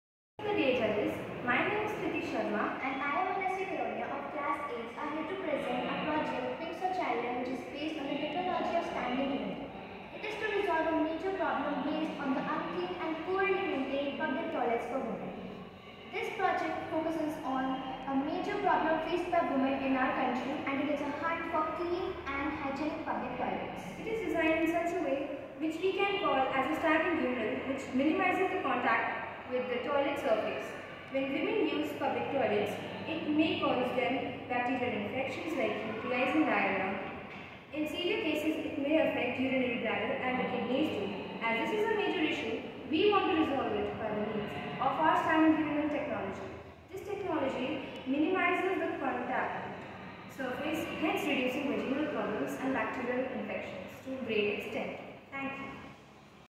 CBSE Science project 'Pink Shauchaalay' by Kriti Sharma and Manasvi Kalonia
Hey everyone...
This video is based on a CBSE Science project named Pink Shauchaalay. It focuses on clean and hygienic public toilet arrangement. It reduces the contact with the surface area hence reducing many bacterial and vaginal infections like Diarrhea or UTIs[ Urinary Tract Infection]. In severe cases,it may end up in damaged kidneys and bladders too.